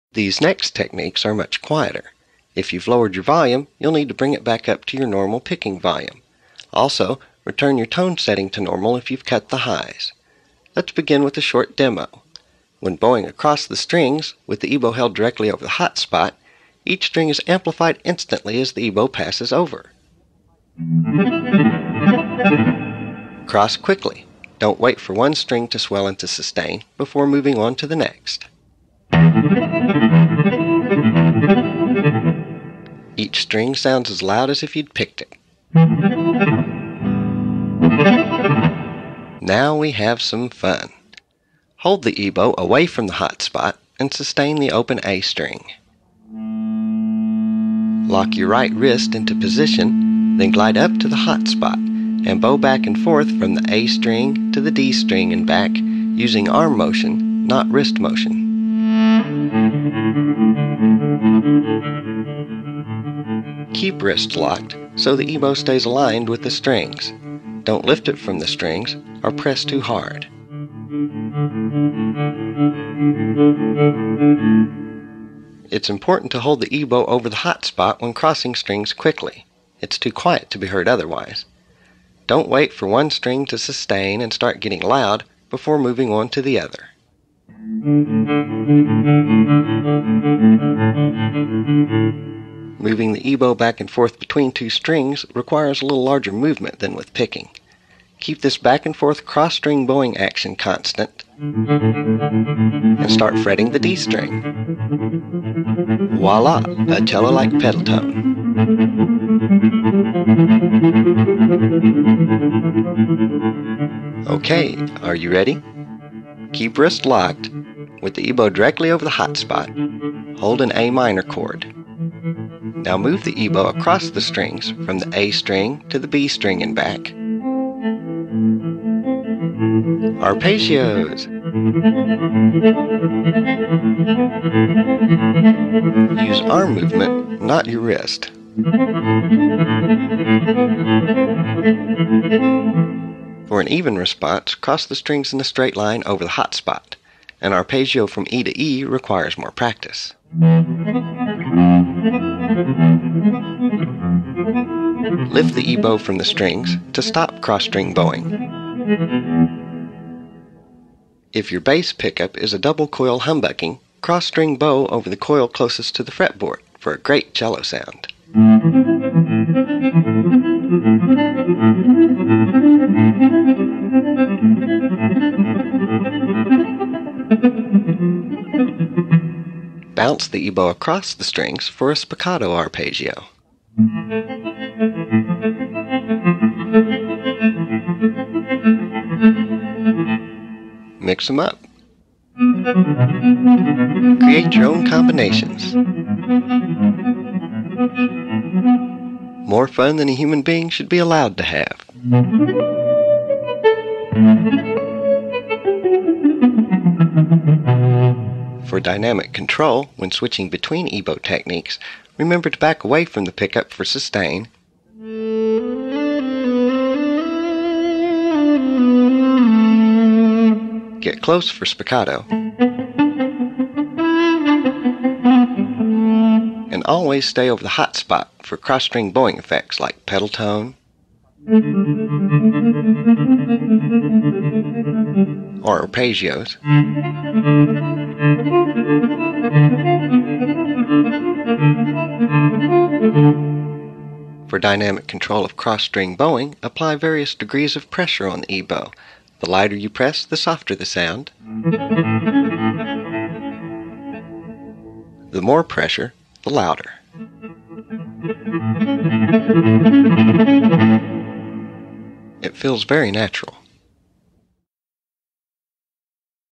EBow Lesson 8. Cross-string bowing - Instant attack - Pedal tone - Cello - Arpeggios
Lesson 8
• Cross-string bowing
• Instant attack
• Pedal tone - cello
• Arpeggios
• Dynamic control